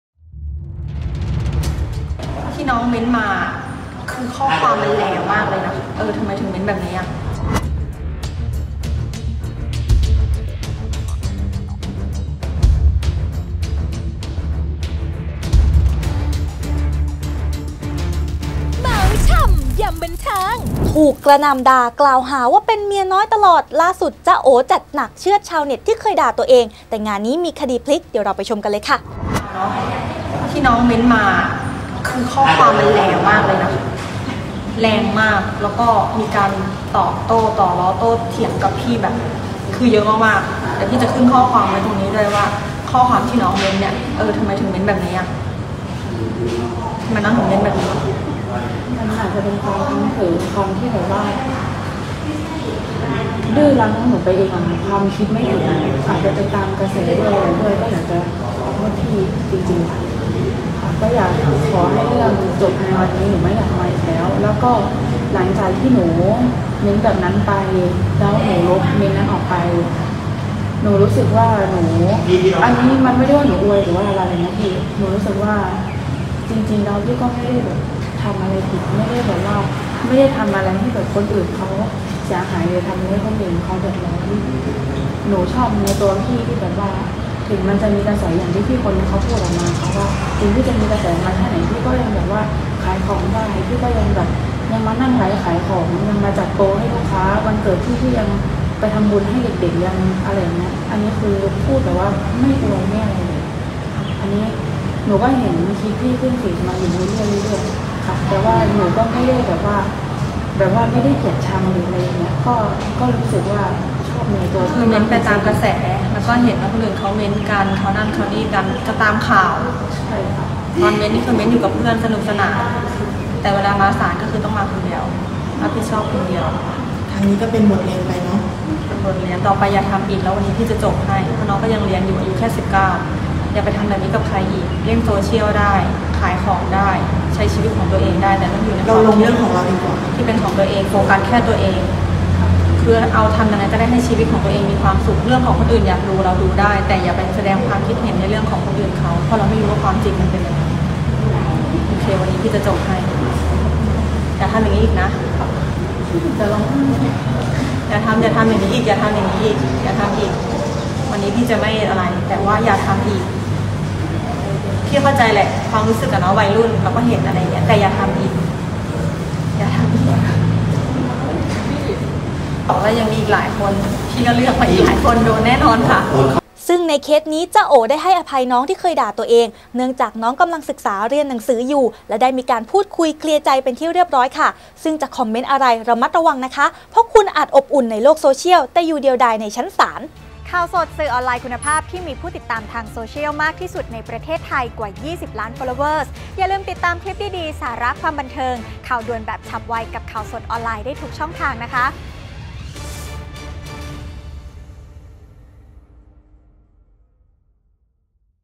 ไม่ยอมแล้ว! “จ๊ะโอ๋” จัดหนัก เชือดชาวเน็ต เคยด่าเมียน้อย งานนี้มีคดีพลิก!! : Khaosod - ข่าวสด
ถูกกระหน่ำด่ากล่าวหาว่าเป็นเมียน้อยตลอด ล่าสุด “จ๊ะโอ๋” จัดหนักเชือดชาวเน็ตที่เคยด่าตัวเอง แต่งานนี้มีคดีพลิก